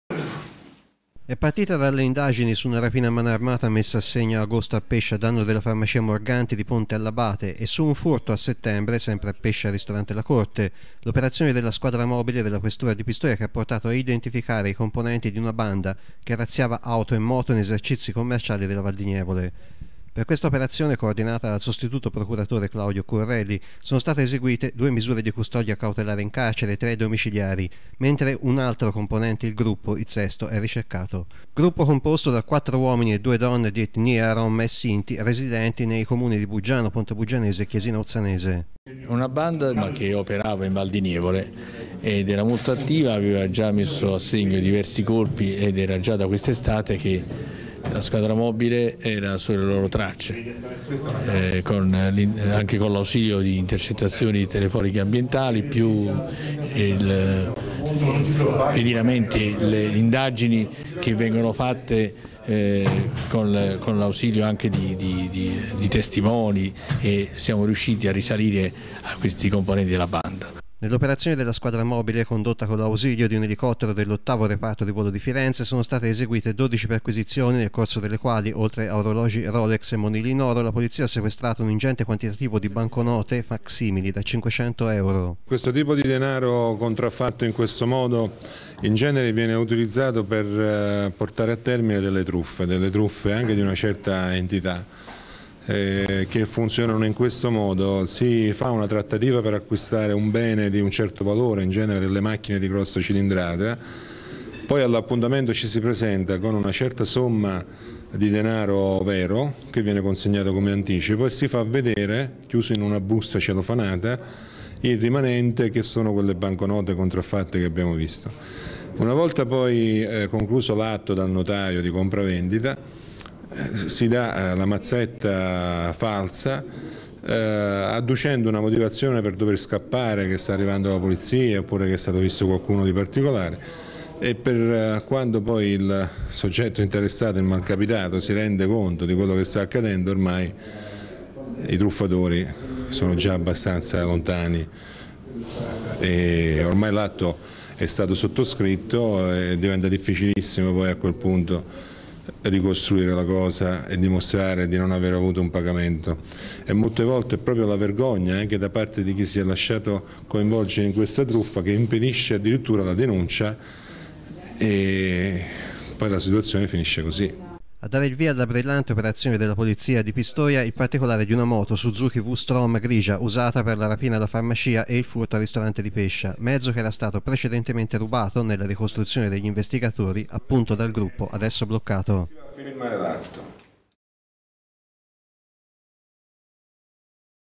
FURTI DI AUTO E MOTO. ARRESTI
La Polizia ha sgominato una banda che commetteva furti di auto e moto in valdinievole